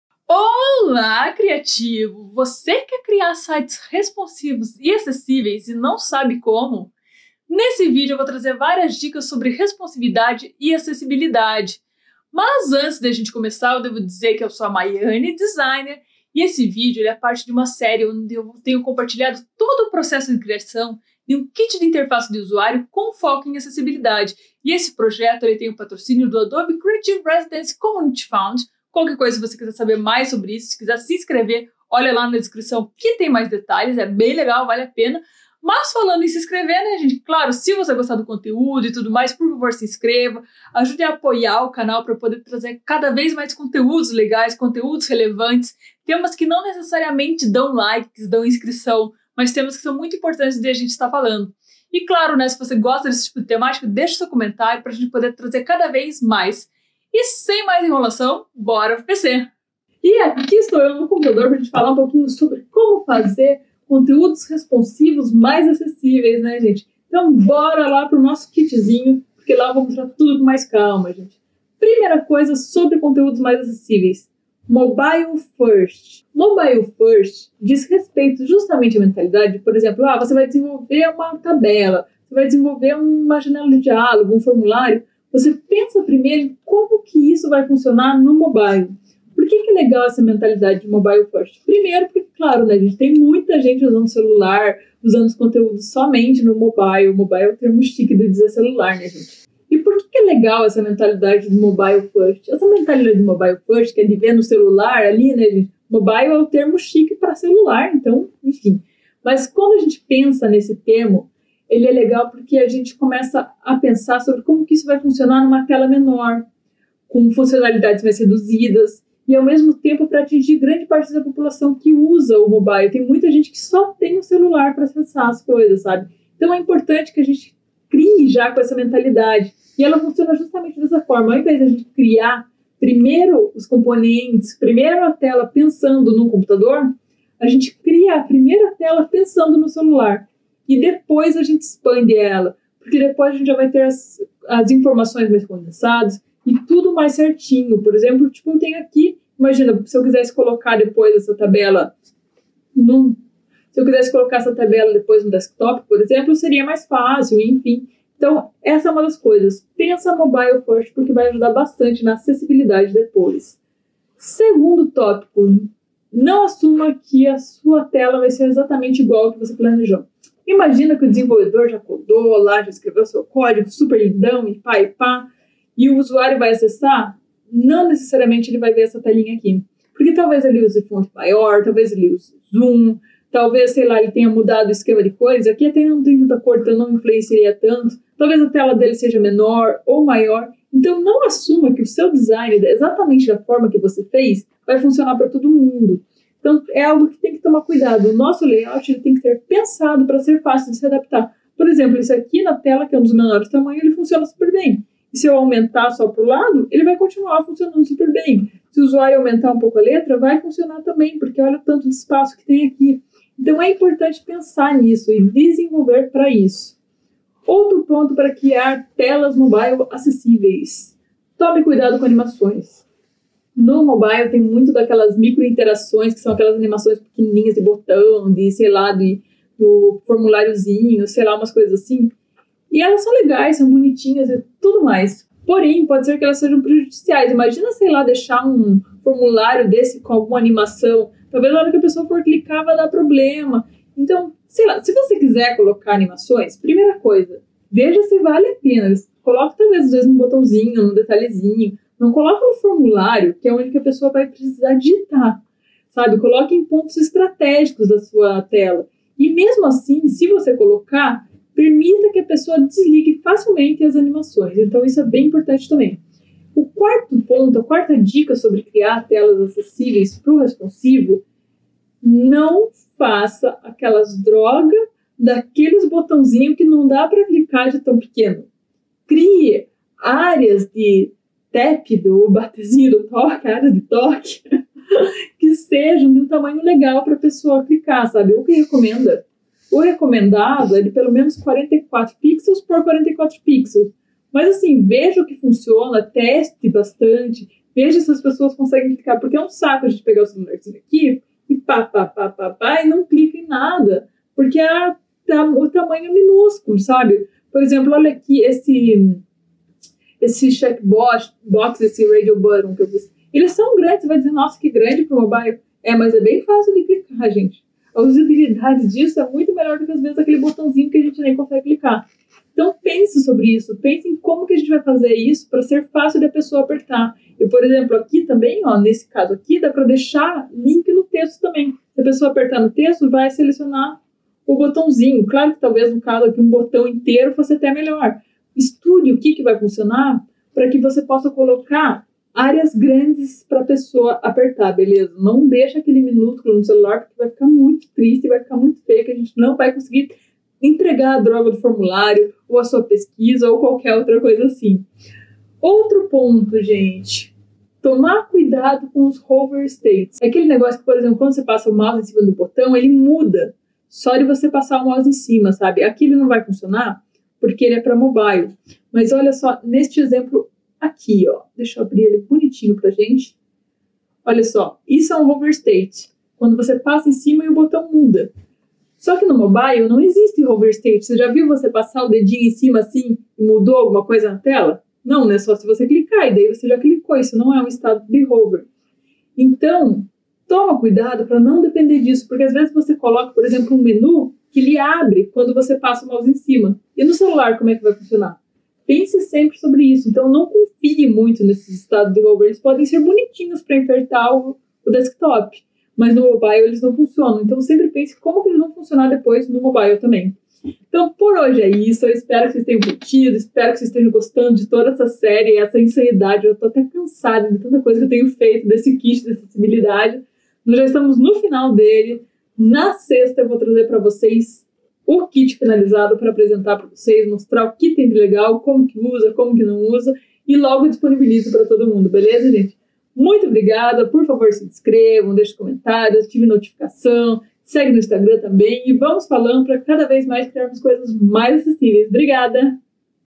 Como criar Layouts Responsivos (mobile) acessíveis| Adobe XD Residency Fund - Parte 11
Você quer criar layouts responsivos e acessíveis? Neste vídeo dou alguma dicas sobre o que você pode fazer para deixar seu design mais inclusivo e melhorar a experiência de todos os usuários.

🐣 - - - - - - - Adobe Creative Residency Community Fund - - - - - - - 🐣

🐣 - - - - - - - DOWNLOAD GRATUITO DO UI ACCESSIBILITY KIT - - - - - - - 🐣

➡ UI/UX Designer Freelancer
➡ Criação de conteúdos
➡ Projetos pessoais
➡ Profissional com propósitos